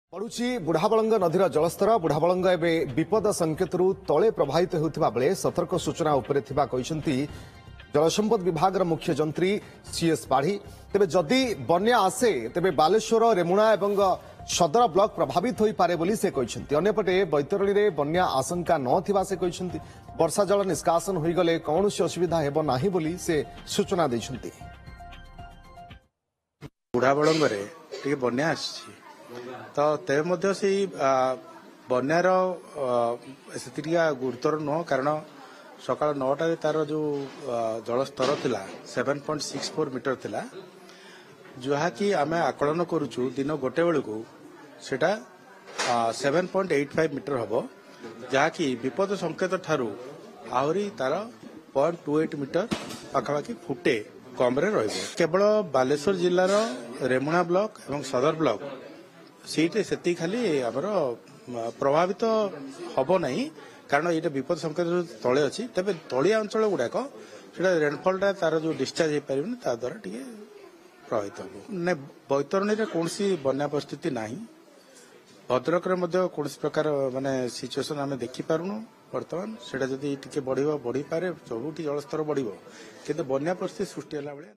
Water Level Rising Above Danger Mark in Budha Balanga River Amid Rainfall in Balasore
ବଢୁଛି ବୁଢାବଲଙ୍ଗ ନଦୀର ଜଳସ୍ତର । ବୁଢାବଳଙ୍ଗ ଏବେ ବିପଦ ସଙ୍କେତରୁ ତଳେ ପ୍ରବାହିତ ହେଉଥିବା ବେଳେ, ସତର୍କ ସୂଚନା ଉପରେ ଥିବା କହିଛନ୍ତି ଜଳସମ୍ପଦ ବିଭାଗ ମୁଖ୍ୟଯନ୍ତ୍ରୀ ସିଏସ୍ ପାଢୀ  ।